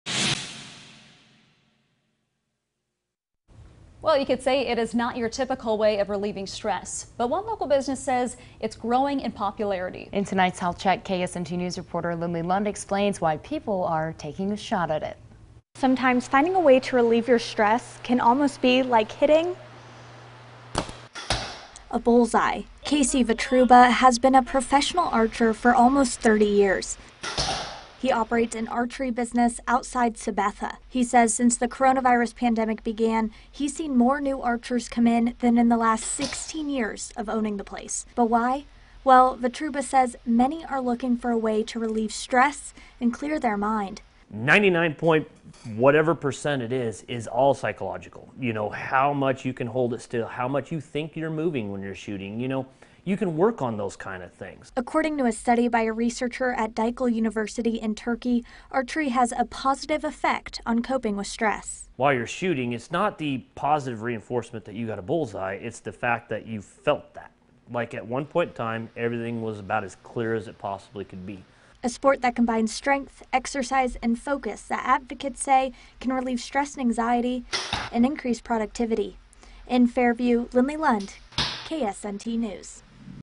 Archery growing in popularity as a way to relieve stress
Finding a way to relieve stress can sometimes be as difficult as hitting a bullseye.